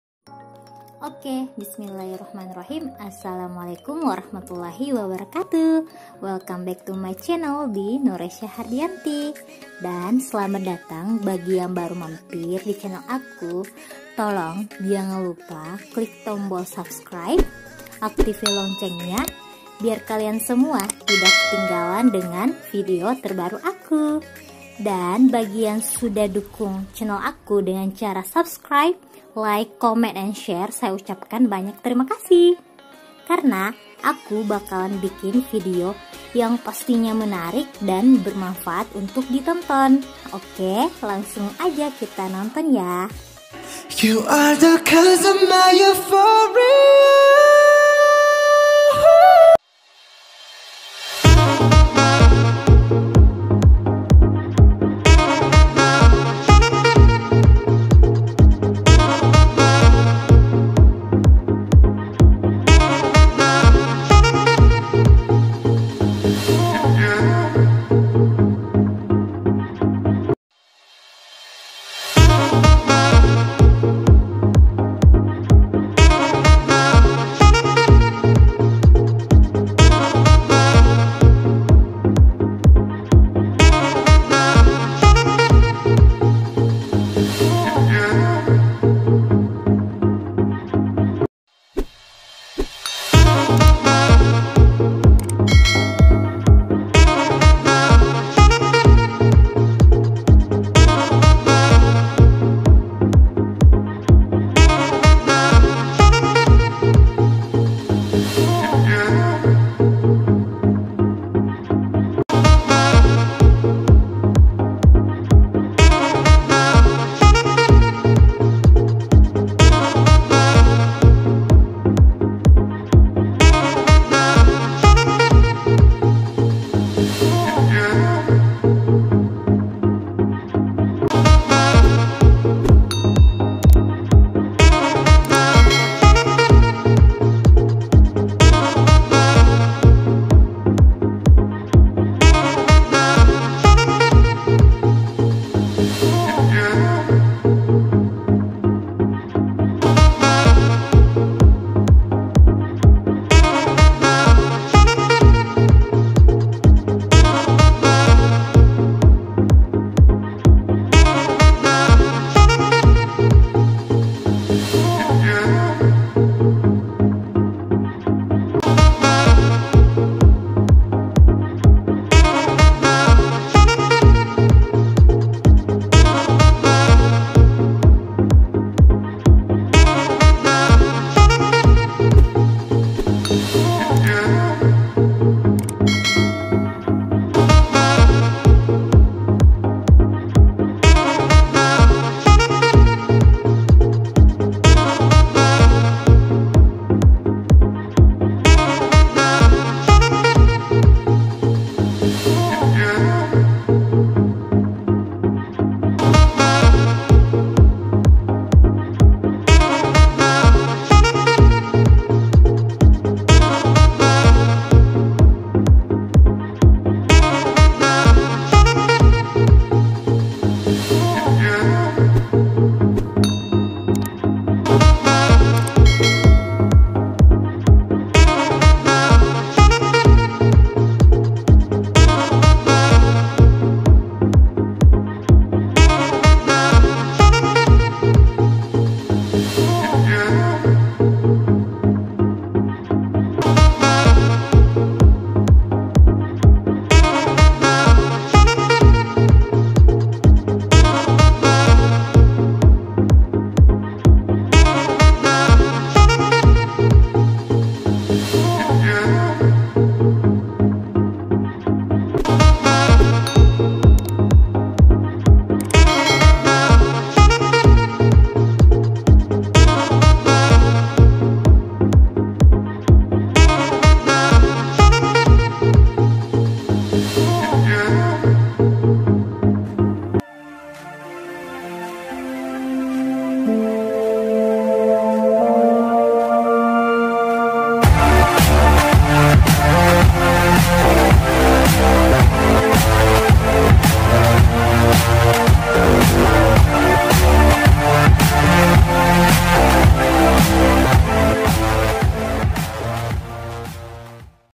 Inspirasi Ootd hijab style remaja warna Dusty pink
ASSALAMU’ALAIKUM SEMUANYA, Bagaimana kabarnya ni? Saya harap kabarnya semua baik-baik saja. merupakan kumpulan slide paduan warna baju dan jilbab yang bisa membuat penampilan kita semakin kece dan terupdate. merupakan kumpulan slide paduan warna baju dusty pink dan jilbab yang bisa membuat penampilan kita semakin kece dan terupdate.

Editor by: Nur Aisyah Hardiyanti

Terimakasih banyak telah menonton, mohon jangan bosan-bosan pantengin video-video saya. Semoga bermanfaat.